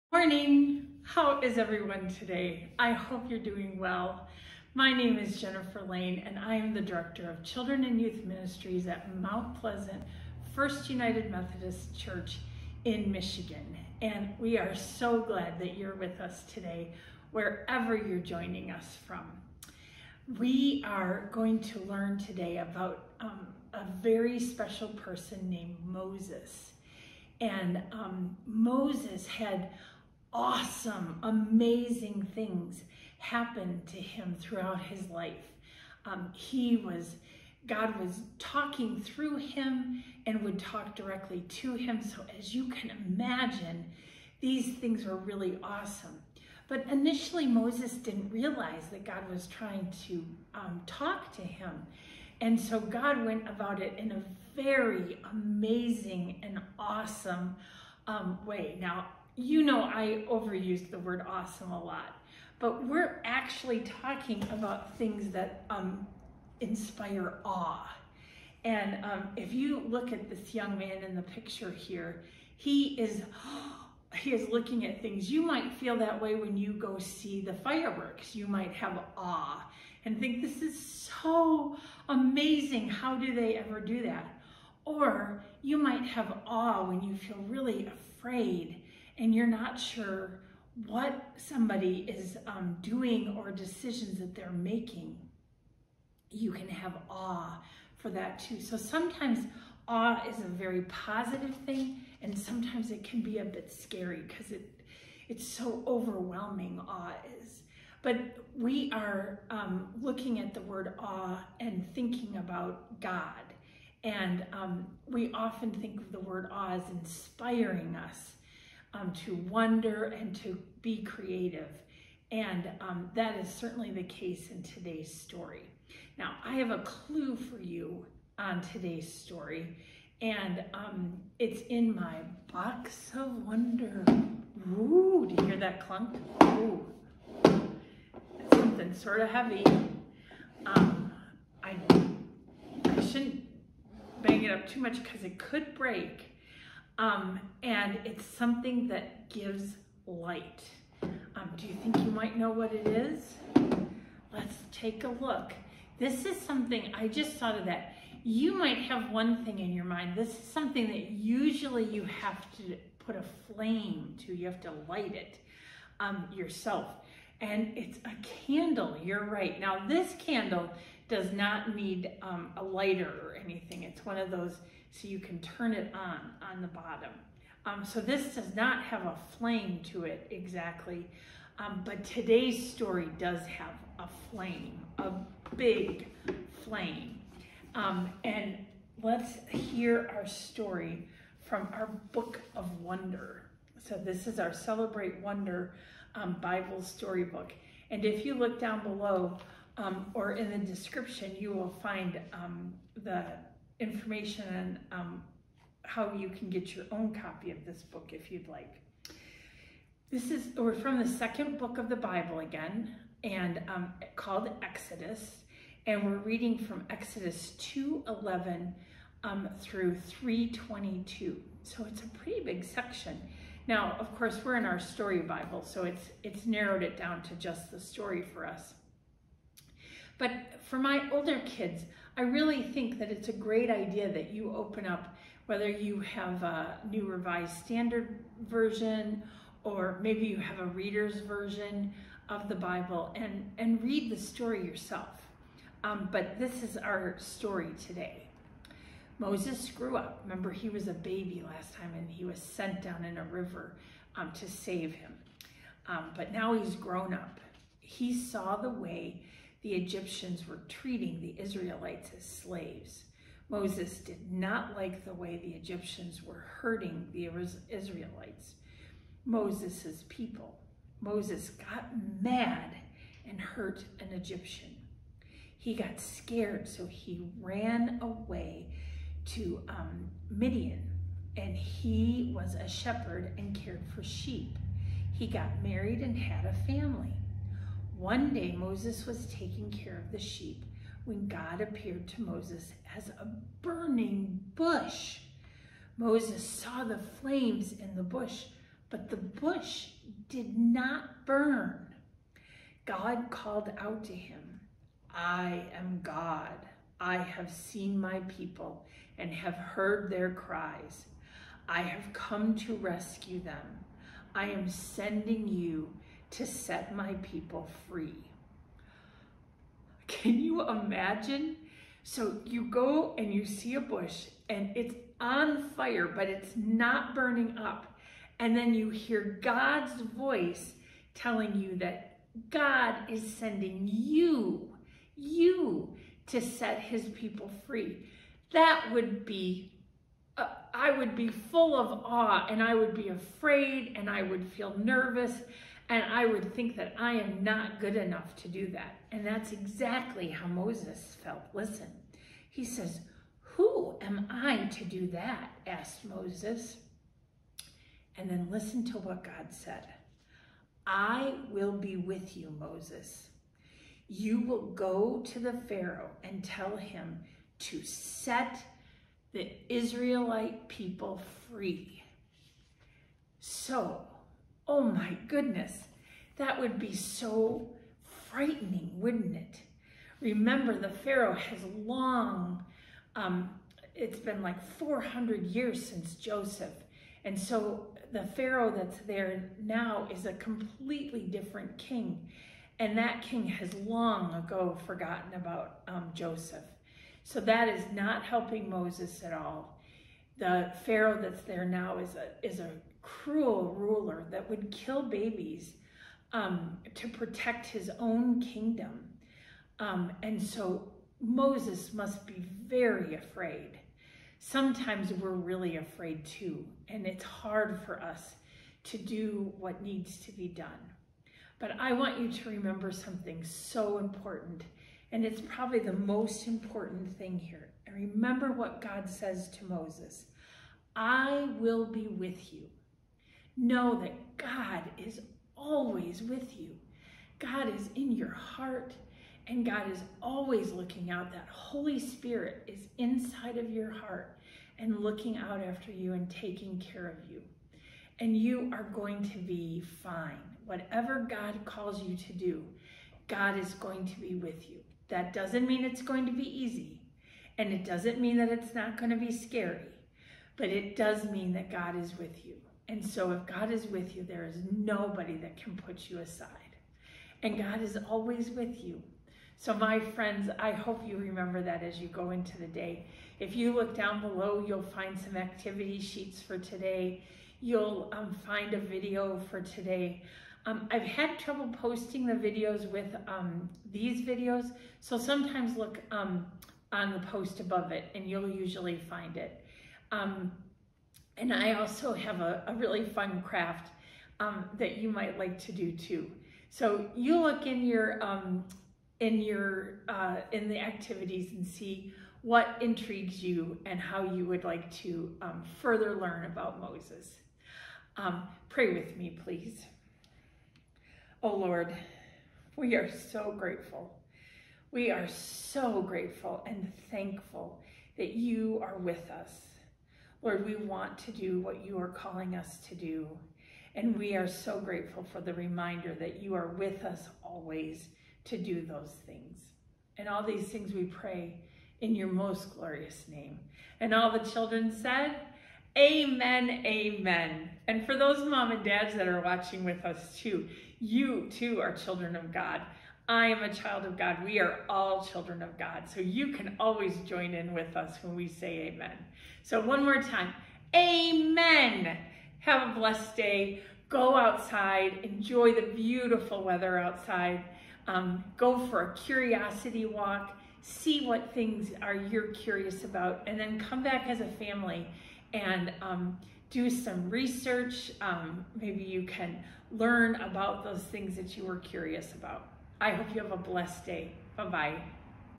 God Speaks to Moses
Come and wonder with Mrs. Jennifer from First UMC in Mt. Pleasant, Michigan, and hear the story of Baby Moses, and how his mother and sister save him. It's an AWEsome story!

This video is made for children preschool to elementary age.

Mrs. Jennifer is reading from her Celebrate Wonder Bible Storybook: The Baby in a Basket, pages 50-51, writer/editor: Brittany Sky, Abingdon Press, Nashville, 2020.